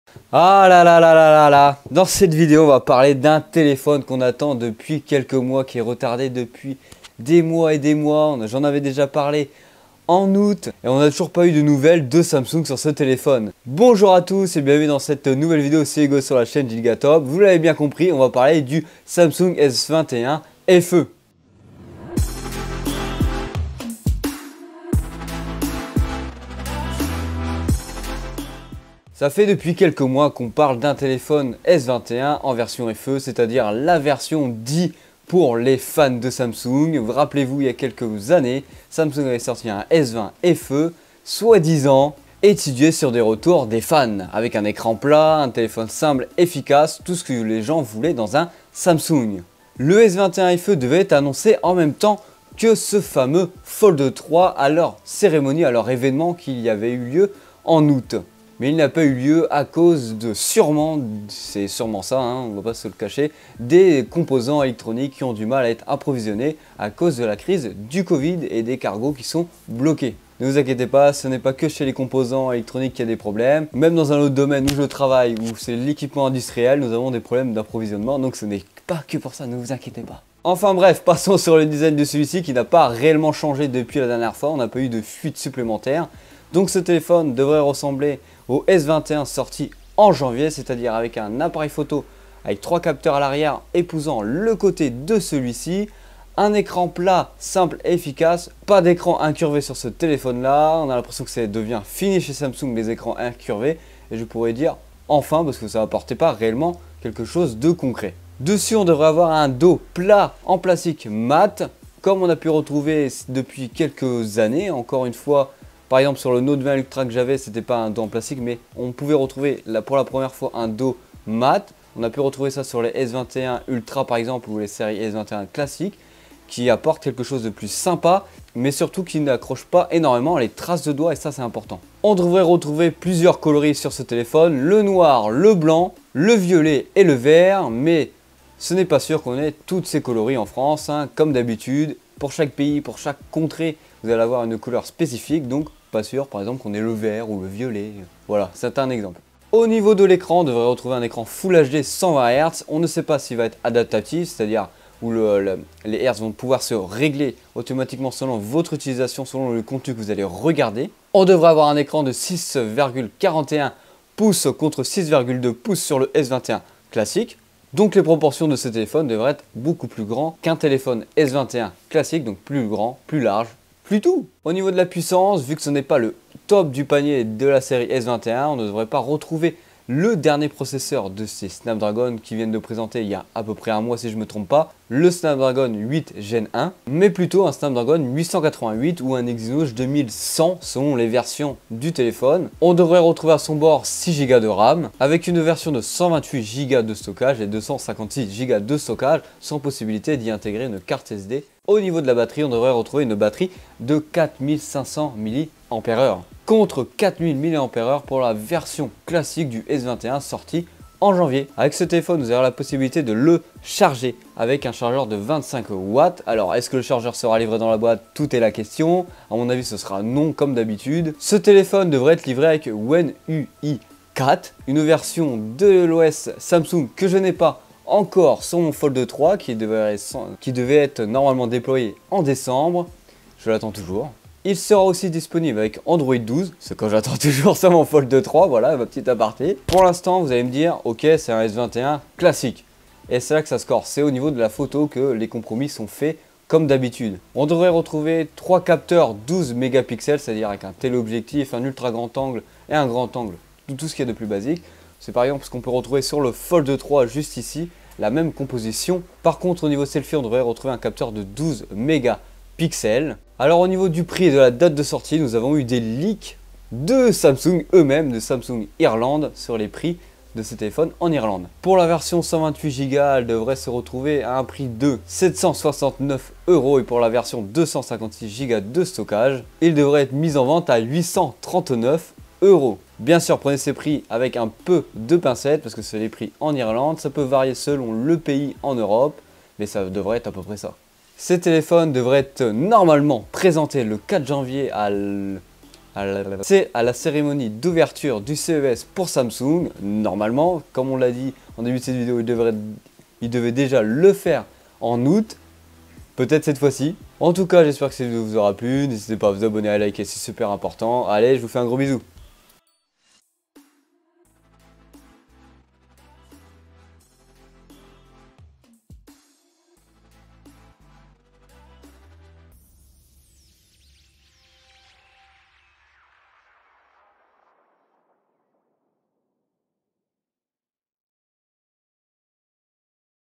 Samsung Galaxy S21 FE - Va t-il réellement sortir en 2022 ? - CES 2022 FR
Dans cette vidéo, je te présente le futur Samsung Galaxy S21 FE, un téléphone attendu depuis plusieurs mois. Il devrait être présenté lors de la cérémonie d'ouverture de Samsung au CES 2022.

00:00 - Introduction
01:20 - Design
02:30 - Ecran
02:45 - Puissance
03:20 - Autonomie
04:00 - Photo
04:25 - Date de sortie & Prix
05:00 - Conclusion